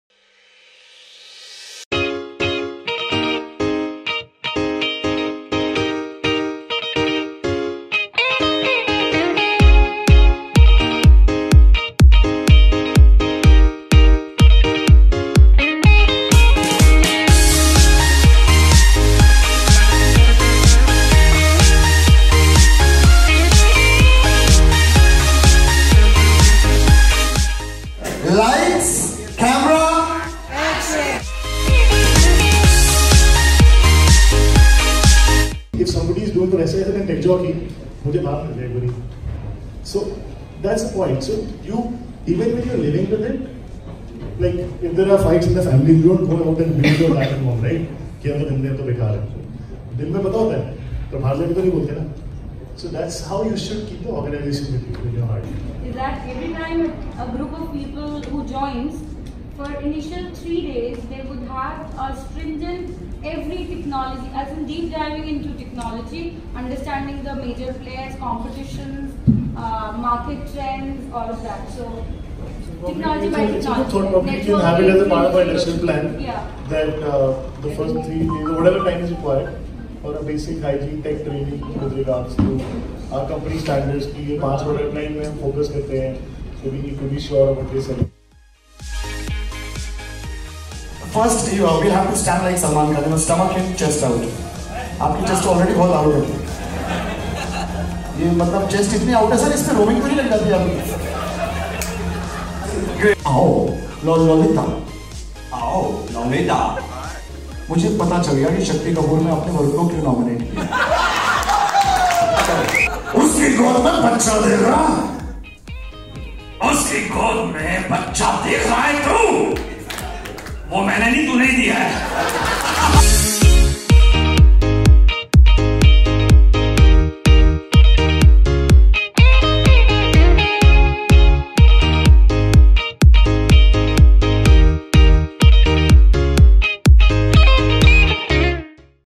Kick Off 6.0 | Techjockey | SISL
Cheers to our annual getaway! Team building activities, brainstorming sessions, and party that continued till the morning! Kick off 6.0 was a success, undoubtedly!